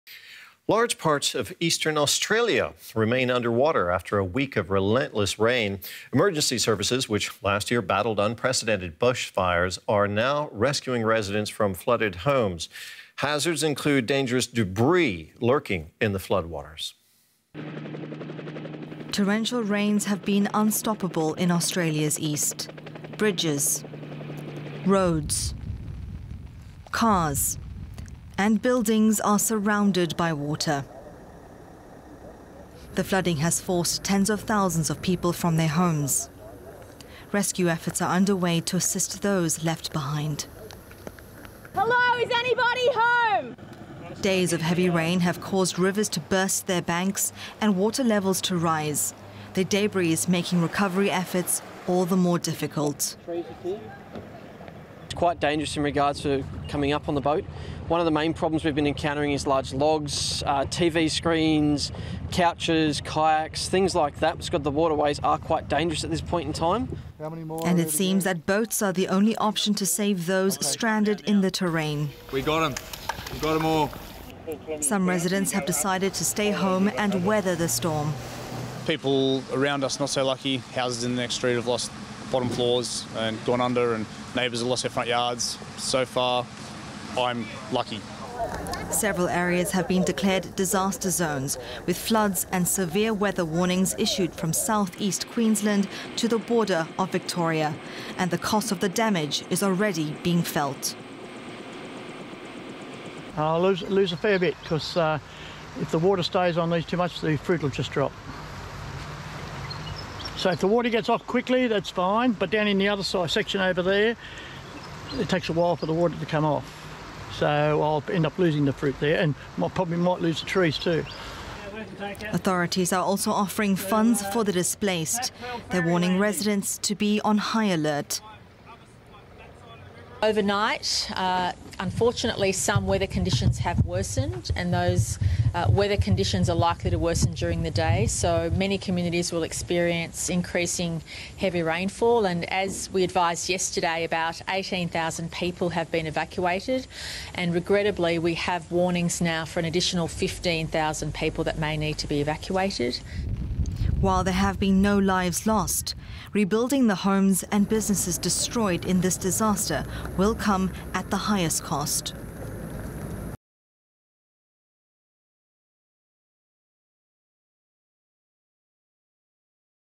Worst flooding Australia has seen in decades | DW News
It’s the worst flooding Australia has seen in decades.

Torrential rains have been unstoppable in Australia’s east.
Bridges, roads, cars and buildings are surrounded by water.

The flooding has forced tens of thousands of people from their homes.
Rescue efforts are underway to assist those left behind.

Days of heavy rain have caused rivers to burst their banks and water levels to rise. And the debris is making recovery efforts all the more difficult.

Several areas have been declared disaster zones…with floods and severe weather warnings issued from South east Queensland to the border of Victoria.